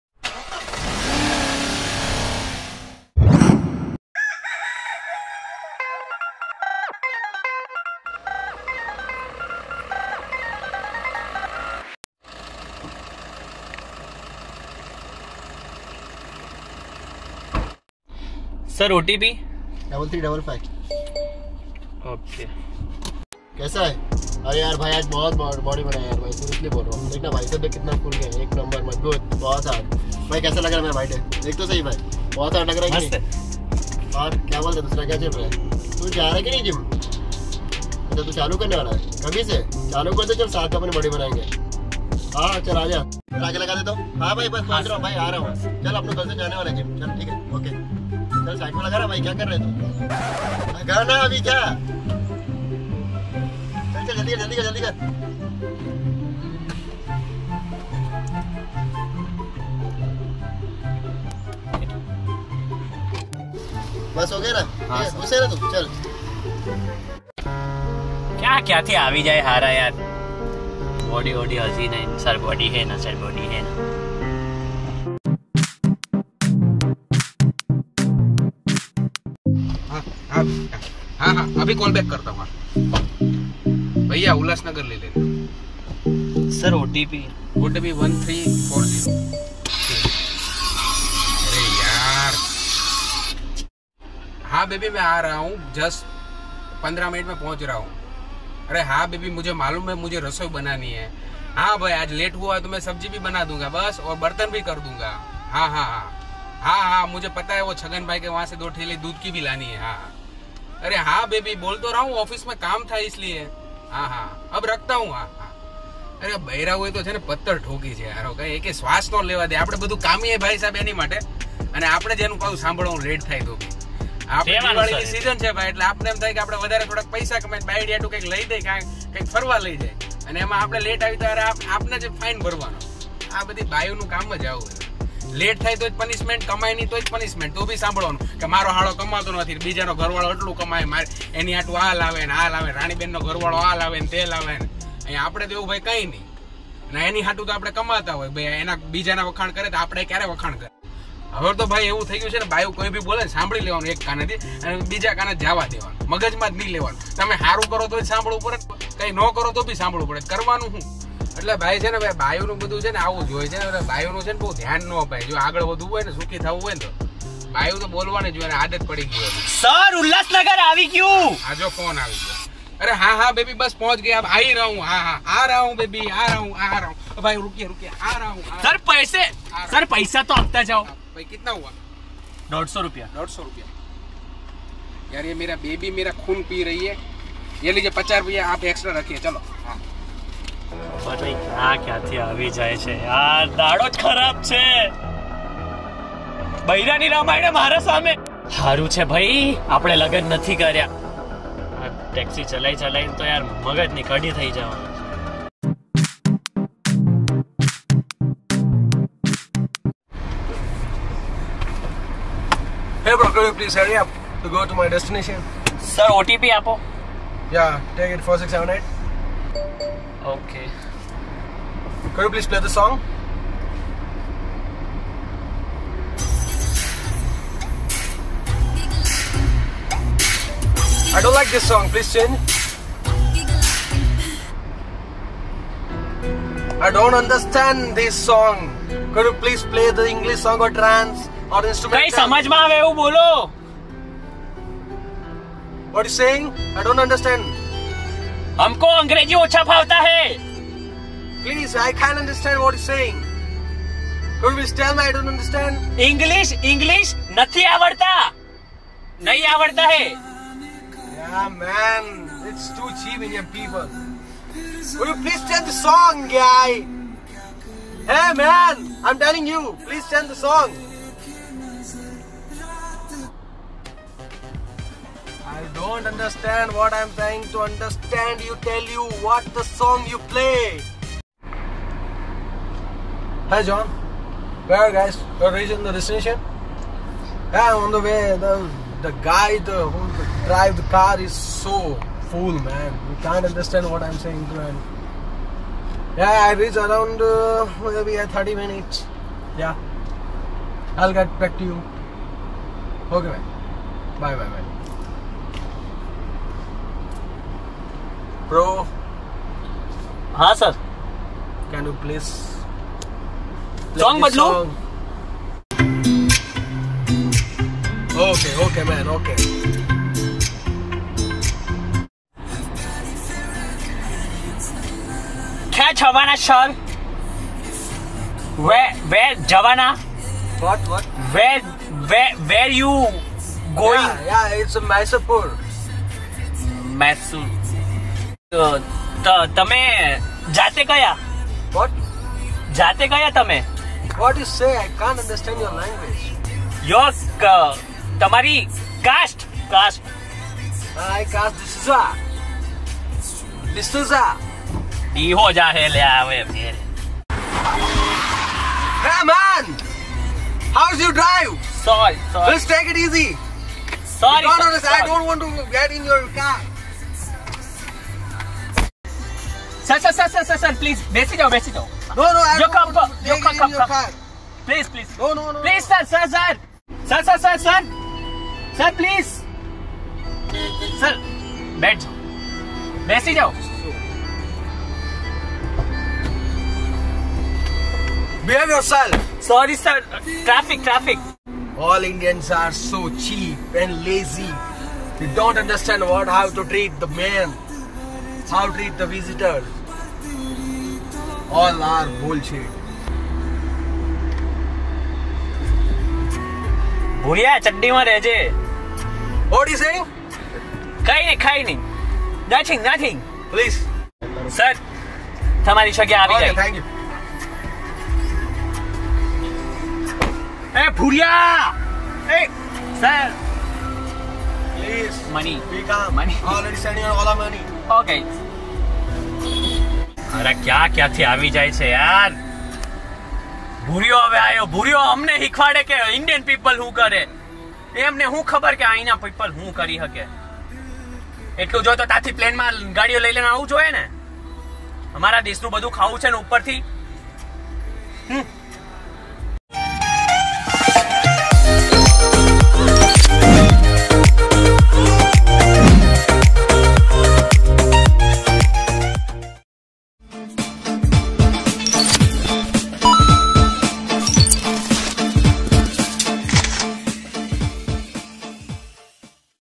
Types of People IN A CAB-TAXI|OLA-UBER
We get to meet all kinds of people in cab from the extremely sweet ones to the creepy ones. Which ones have you come across?

We are back with another video for you all. Hope you all enjoy Nd  like this video.

1)@sumantahir
2)nileshpanchal_official

bell icon button also to get notifications of interesting videos from Sumant Ahir
CAST
Nilesh Panchal
Himanshu bhrambhatt
Dinesh Yadav
Sumant Ahir